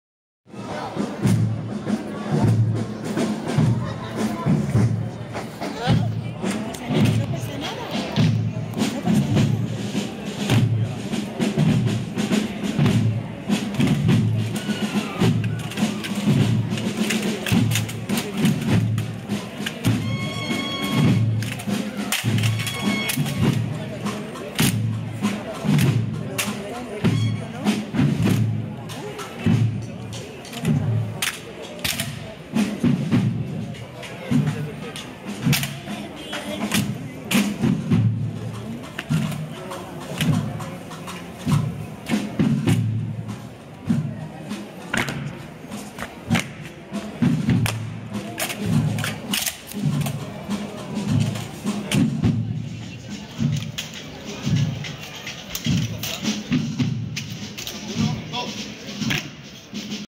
La legión alhama de Murcia
Aquí los dejo un vidio de recuerdo de la legión 2019 para recordar esa buena presencia 😎